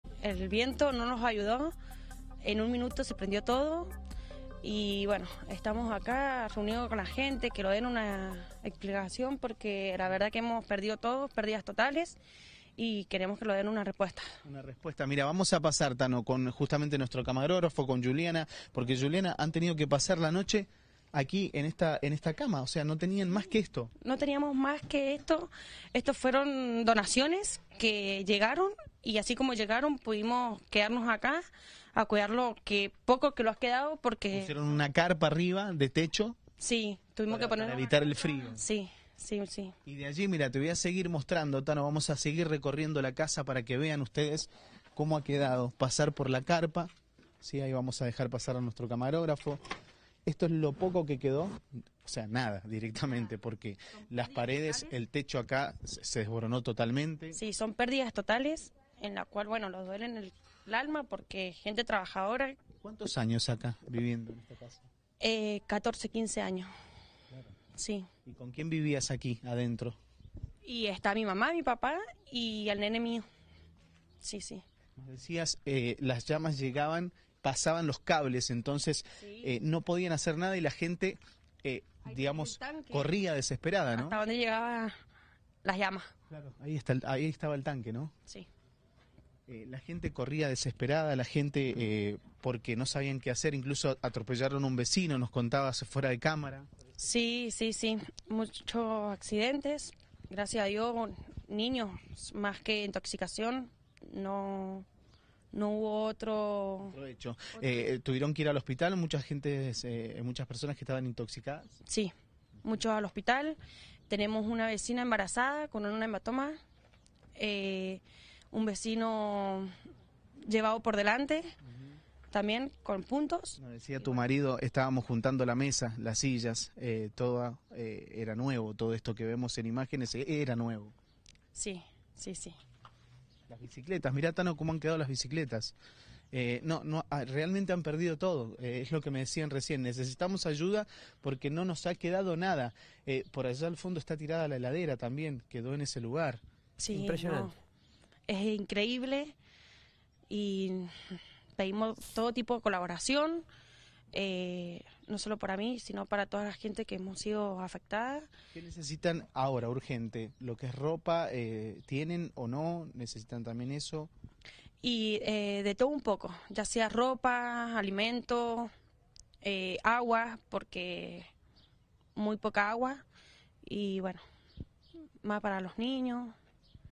👉Las PÉRDIDAS por el INCENDIO en la PLAYA SAN AGUSTÍN
Un incendio de grandísimas proporciones que se generó durante la madrugada de este sábado en la playa de secuestro de vehículos de San Agustín, perteneciente a la Policía de Mendoza y al municipio de Capital, produjo pérdidas totales en 4 casas y parciales en otras 15 del barrio La Favorita.